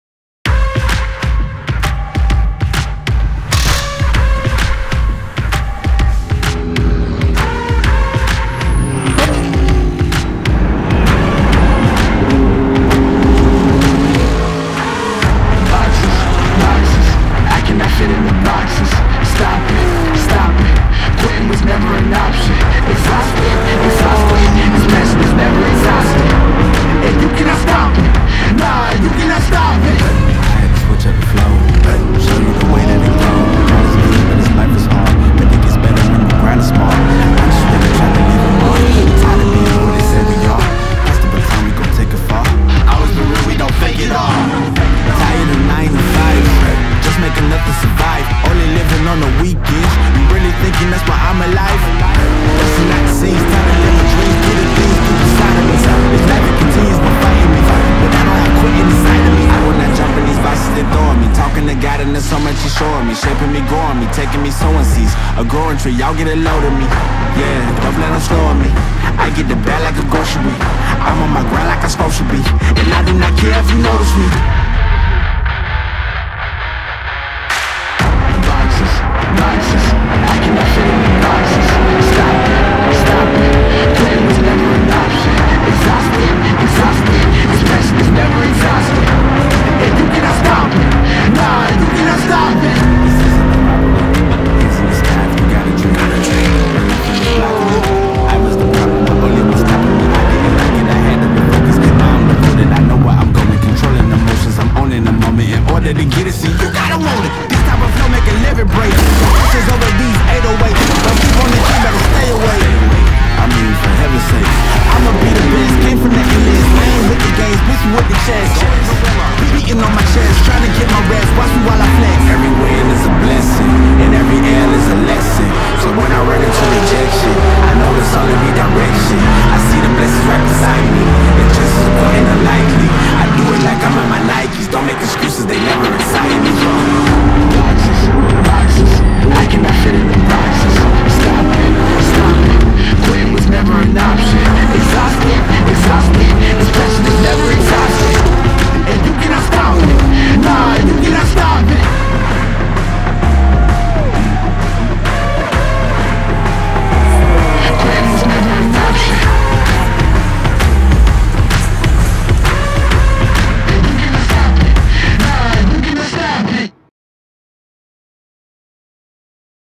Cinematic Highlights l Fanatec GT World Challenge America powered by AWS - Sebring 2023
We respected the bumps at Sebring International Raceway for the penultimate round of Fanatec GT World Challenge powered by AWS. Relive the action with our cinematic highlights.